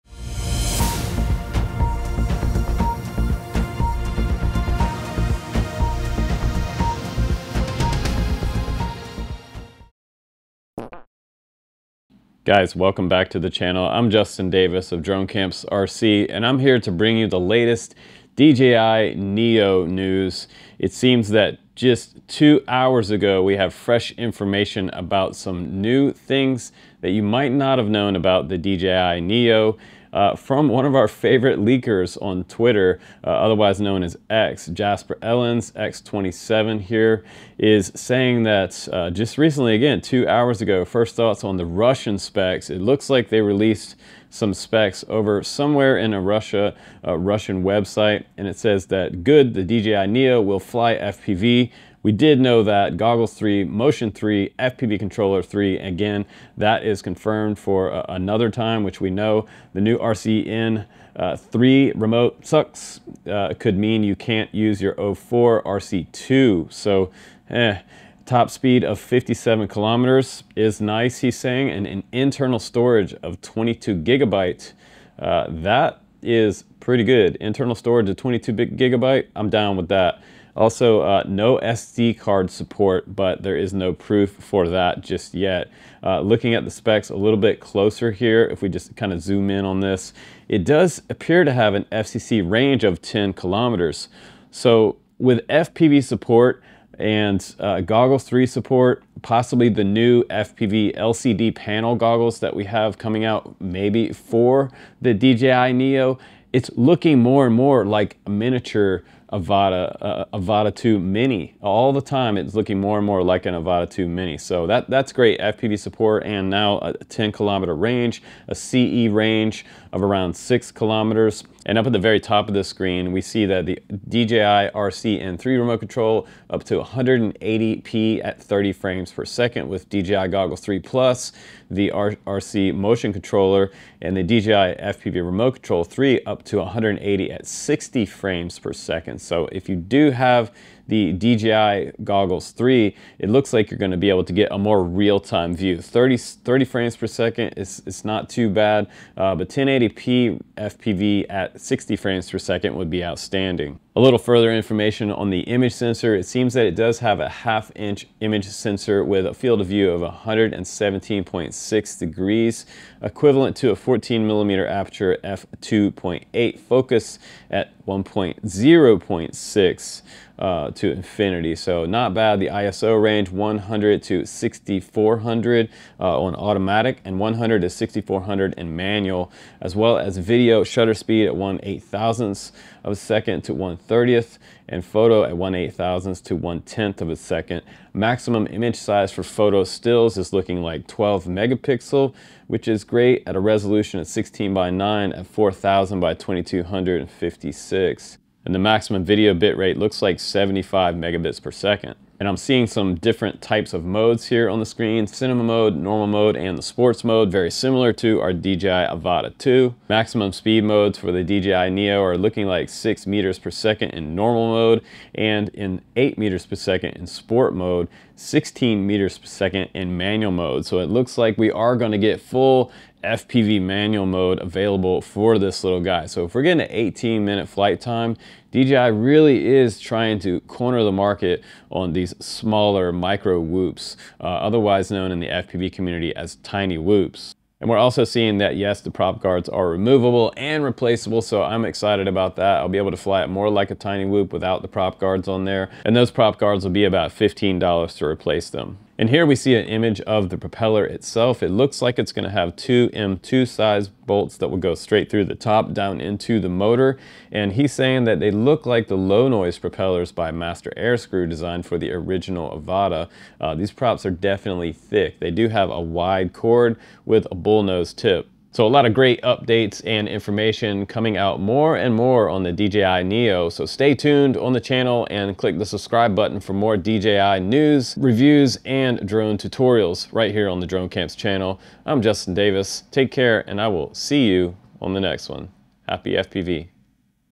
NEW LEAKS!!! - DJI NEO 10KM Range & More!!!
DJI Neo Leaks and Updates. Great News!!! 10km FCC Range, 6Km EU Range, 1/2" 4K Image Sensor, 22Gb Storage, and more!!!

$2 a month if you like the content.

T-Shirts, Long Sleeve, Zip Hoodies, and more